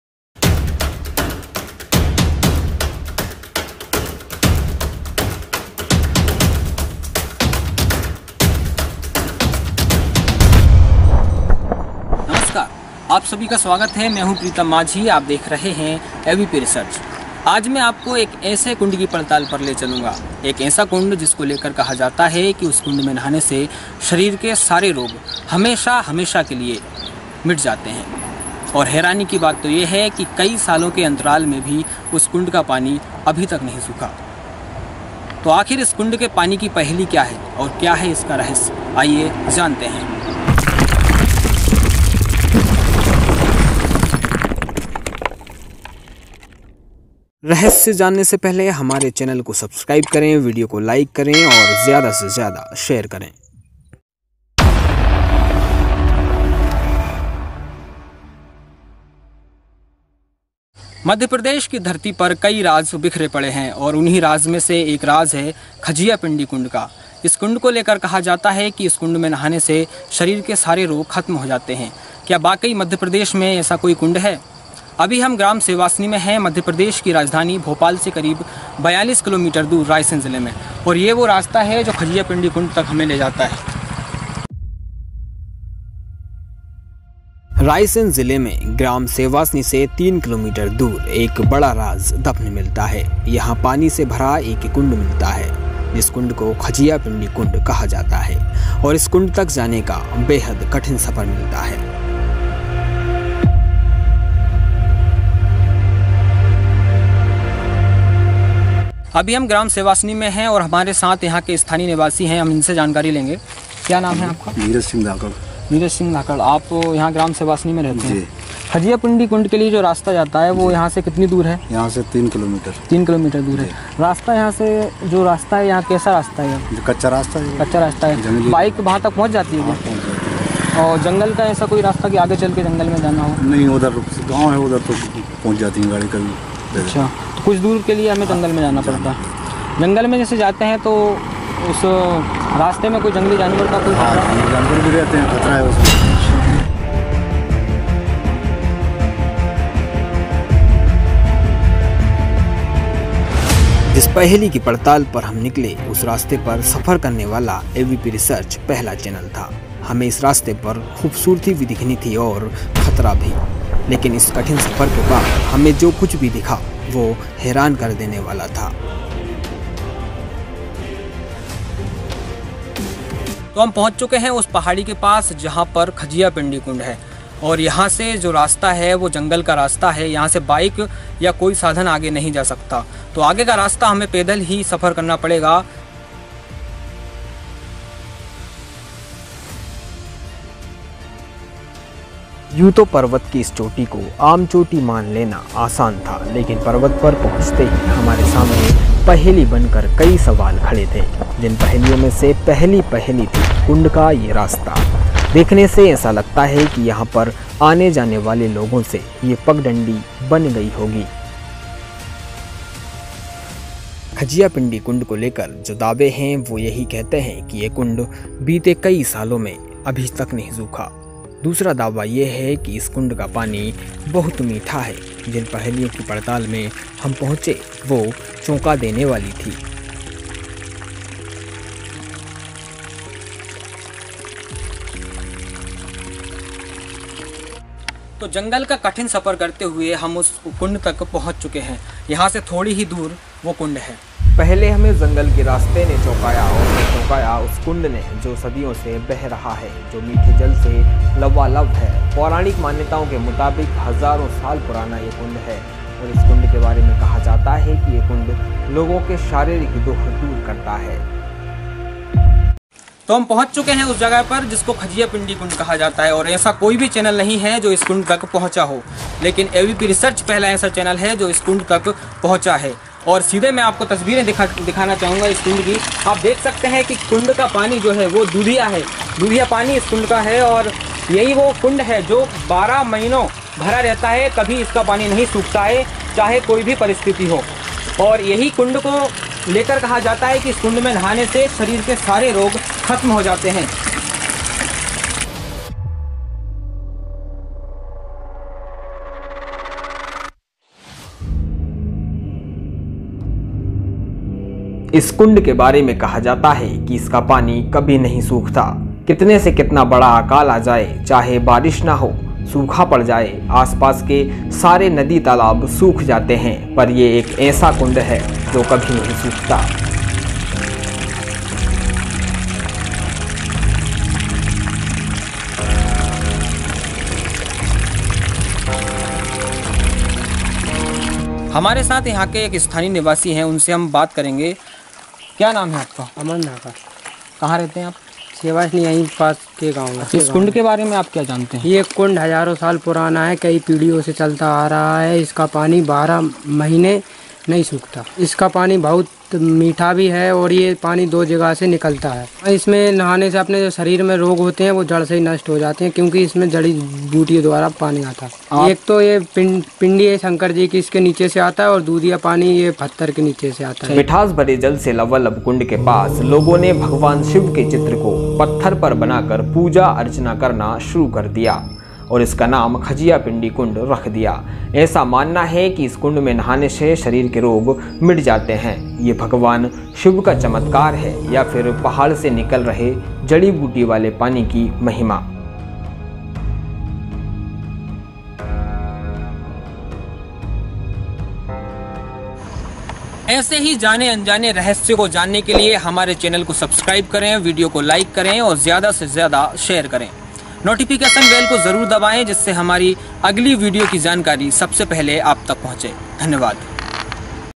शरीर के रोग मिटाने के लिए एक बार यहां जरूर आए । खजिया पिंडी कुंड
शरीर के रोग मिटाने के लिए एक बार यहां जरूर आए

खज़िया पिंडी कुंड एक ऐसा कुंड है...जिसमें नहाने मात्र से शरीर के रोग मिट जाते हैं...क्योंकि कुंड का जो पानी है...वो जड़ी बूटियों वाला पानी है...जो जंगल की चोटी से निकल रहा है...दूधिया पानी है... और पीने में बहुत मीठा लगता है...कुंड को लेकर मान्यता है... कि कुंड का पानी 12 महीनों के अंतराल में कभी नहीं सूखता... चाहे कोई भी परिस्थिति क्यों ना हो...और ये कुंड मध्यप्रदेश की राजधानी भोपाल से करीब 42 किलोमीटर दूर  ग्राम सेवासनी के पास है... जो कि रायसेन जिले में है...!!

AVP RESEARCH

नोट - और भी रहस्य देखने के लिए नीचे दिए गए लिंक पर जाए...👇🏻👇🏻